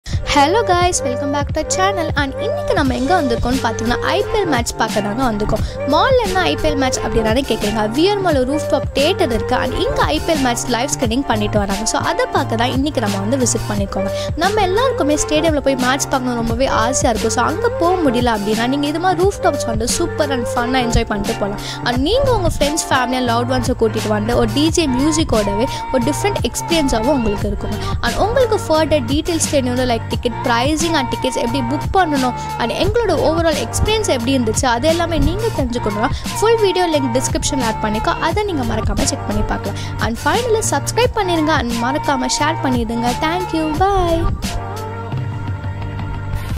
Rooftop Cinema in VR Mall 🎥 | VR mall ல Open theatre இருக்கா 🎬| CSK VS MI IPL Fan Park 💛💙 | Skydeck
🎥Rooftop cinema carnival
Movie Theatre
Ultimate Open-Air Theatre in Chennai,
🍿Themed Food Menu, Premium Beverages & Popcorn,
🎬Cult Films, Classics, People's Favourite

📍🌏Location
100 Feet Rd, Thirumangalam, Anna Nagar, Chennai, Tamil Nadu 600040

Ticket rate 200 Rs

PC,VE-Vignesh
EC-Moni

🔔Turn on the notification for new updates

love Status
love_feel_whatsapp_status in Tamil
Couple😘Statuz_Tamil
Couple😘Status_Tamil